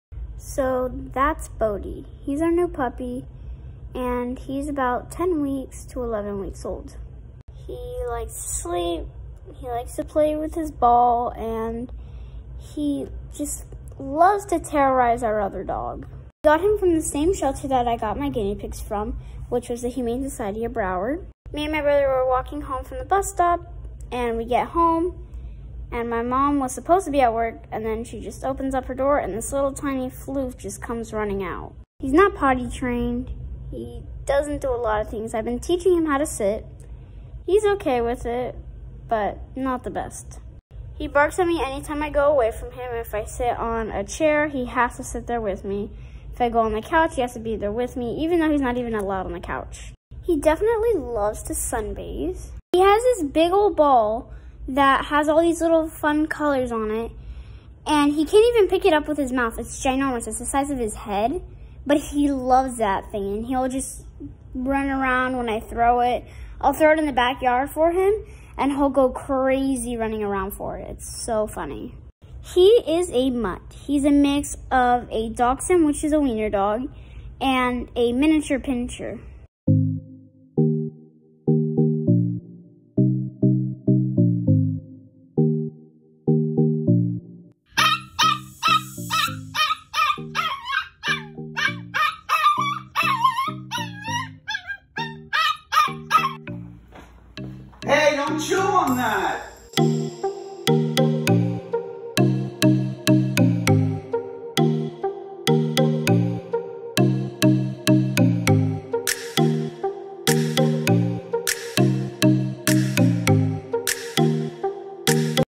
Meet out New Pet (very cute)
Today you meet our new pet his name is Bodhi. We have had him for 16 days.